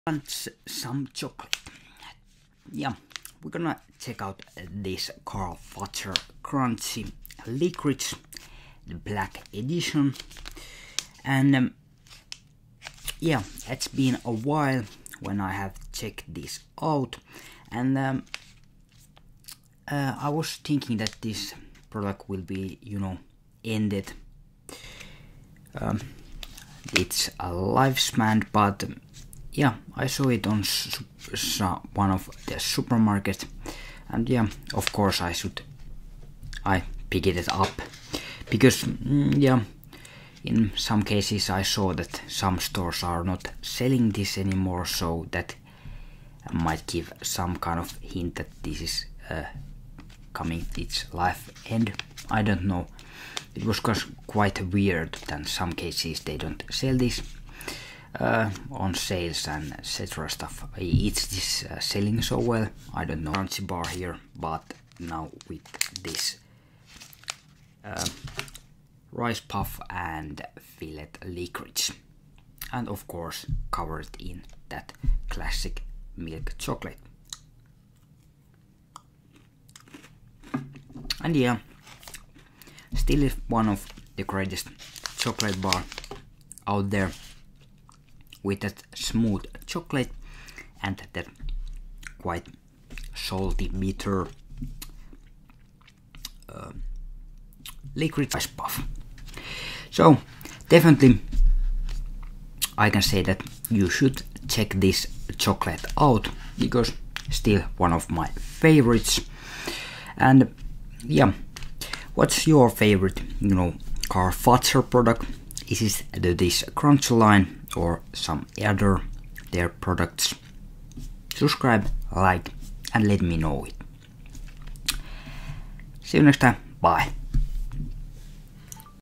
Back From the Dead: Can the Crunch Back Edition Live Up to the Hype?
Taste test of Fazer cruch Black edition.